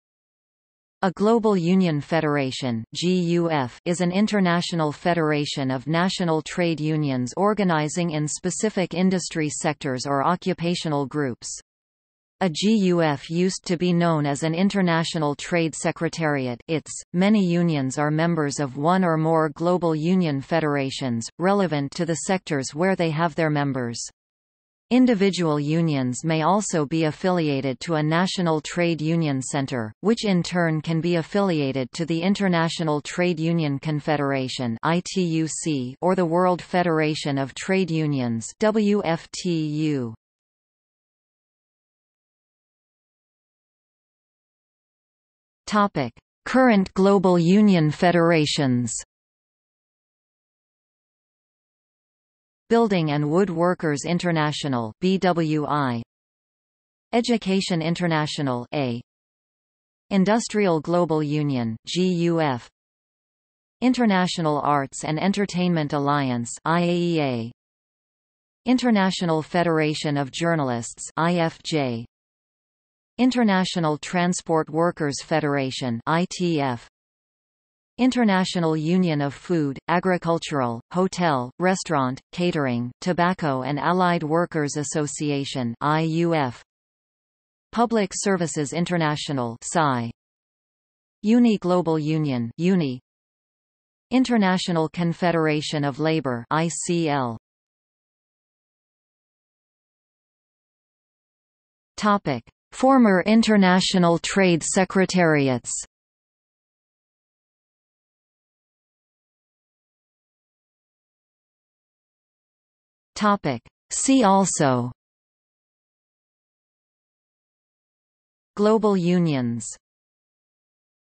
Global union federation | Wikipedia audio article
00:00:42 1 Current global union federations
00:01:38 2 Former international trade secretariats
00:01:49 3 See also

- Socrates

SUMMARY
A global union federation (GUF) is an international federation of national trade unions organising in specific industry sectors or occupational groups. A GUF used to be known as an international trade secretariat (ITS).Many unions are members of one or more global union federations, relevant to the sectors where they have their members. Individual unions may also be affiliated to a national trade union center, which in turn can be affiliated to the International Trade Union Confederation (ITUC) or the World Federation of Trade Unions (WFTU).